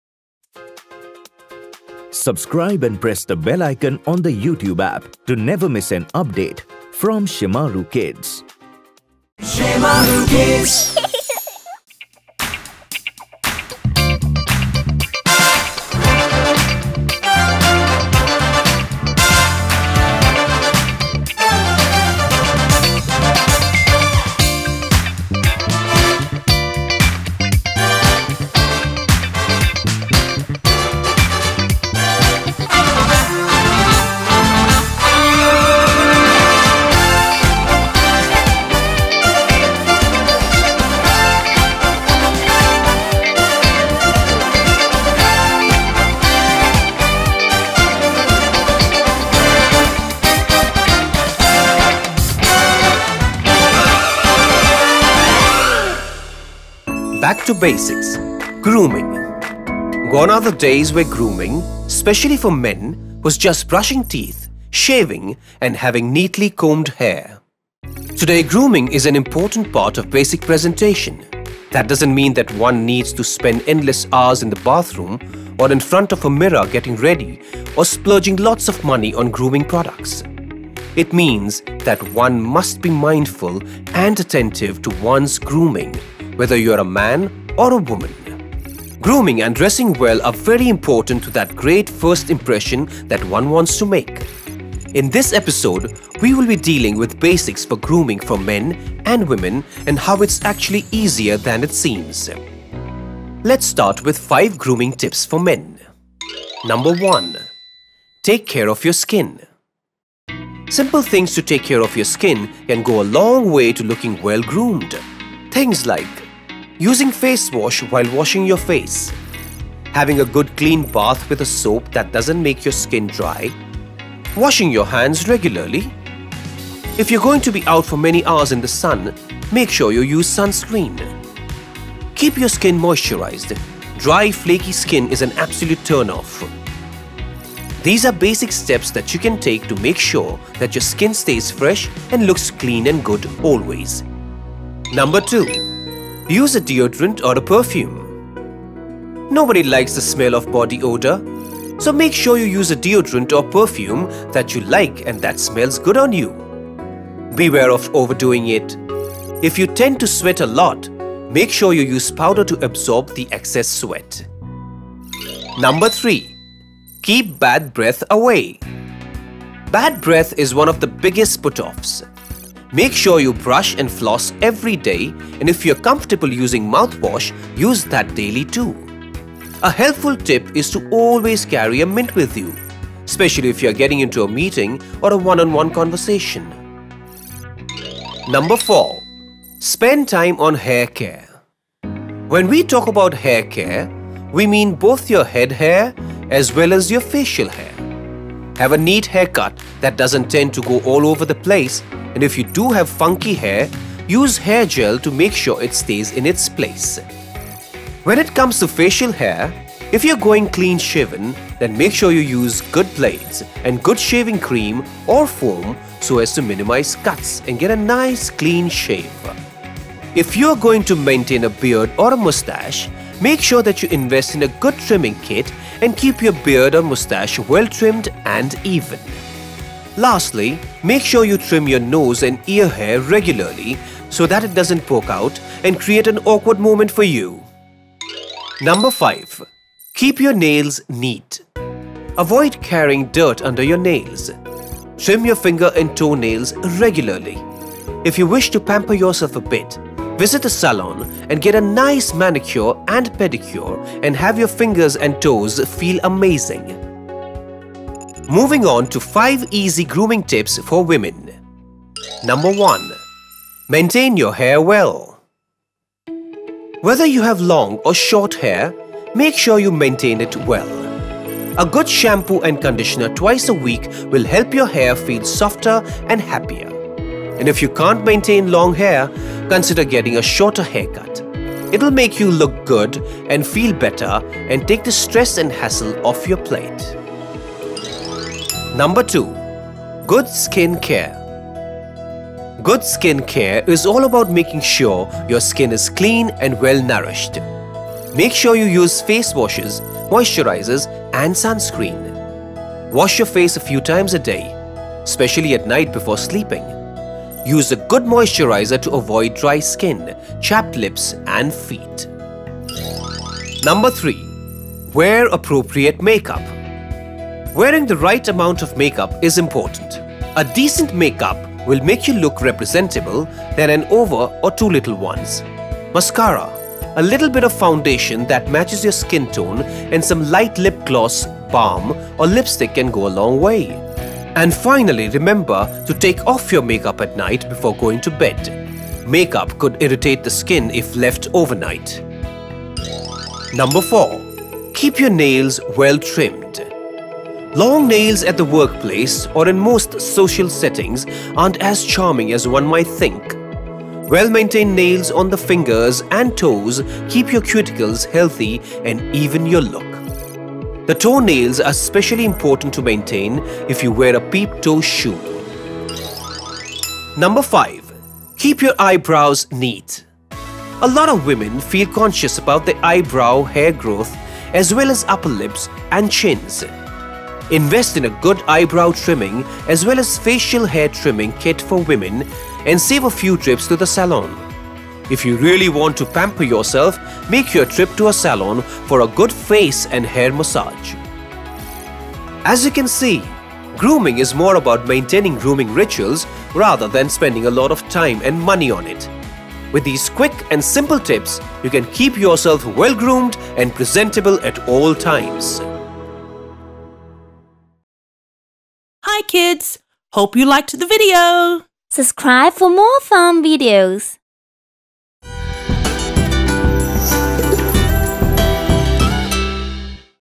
Back to Basics | Grooming & Personal_Hygiene | Shemaroo Kids
Shemaroo Kids presents to you, “Back To Basics” a collection of self-education videos.

As a part of “Back To Basics” here is a video about grooming and personal hygiene. This video will handhold you to the rudimentary tricks of grooming and help you maintain your personal hygiene. It also stresses the importance of grooming and lets you know the basics of it so that you are able to lead a better lifestyle.